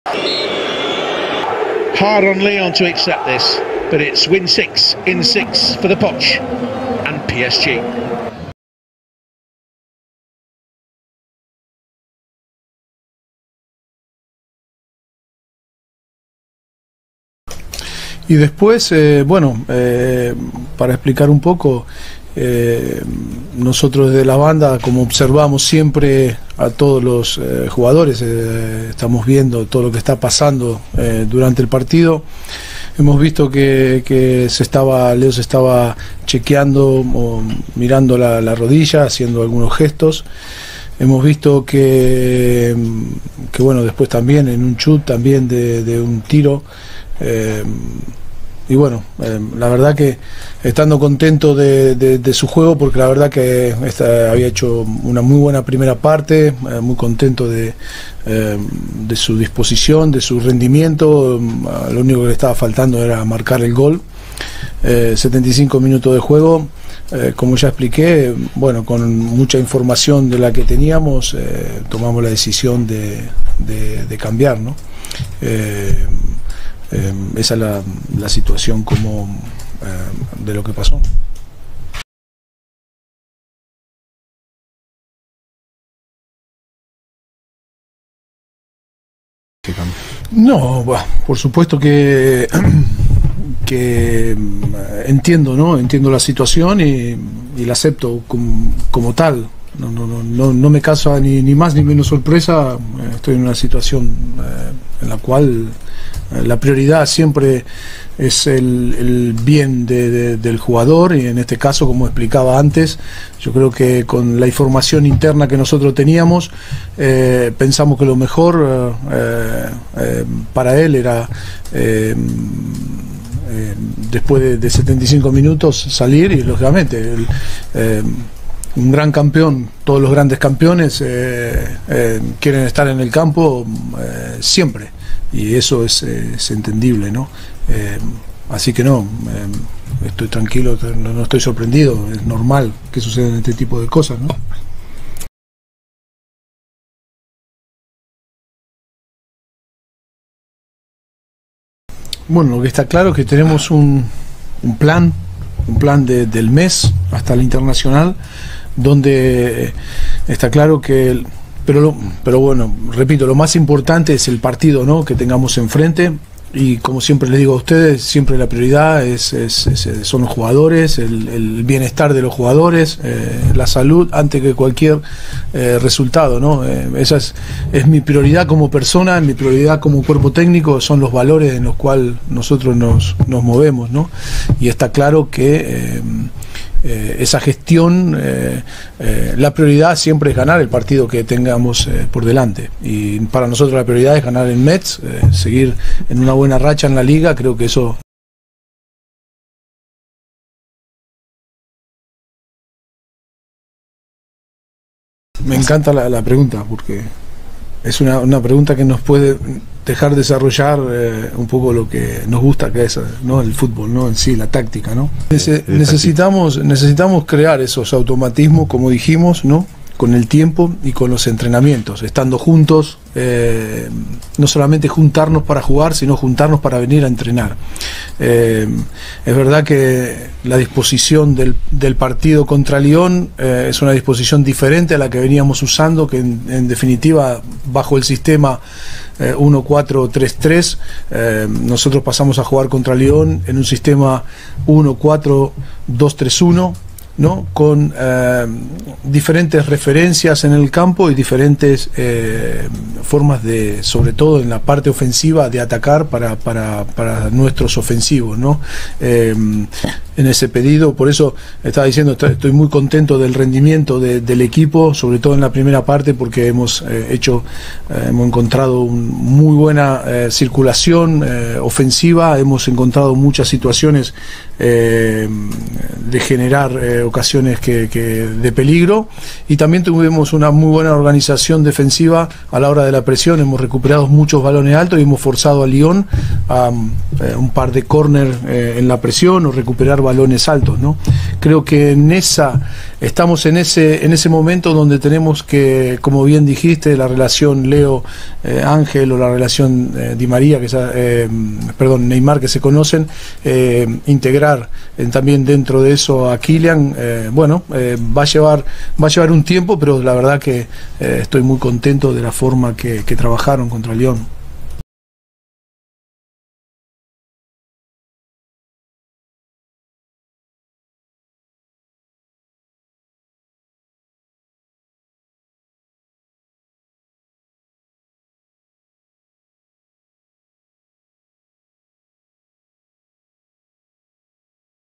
MESSI CEDERA - FC METZ VS PSG - PRESS CONFERENCE
Messi cedera lutut kiri, memar pada tulang - info yang disampaikan PSG. Press conference Metz vs PSG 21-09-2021.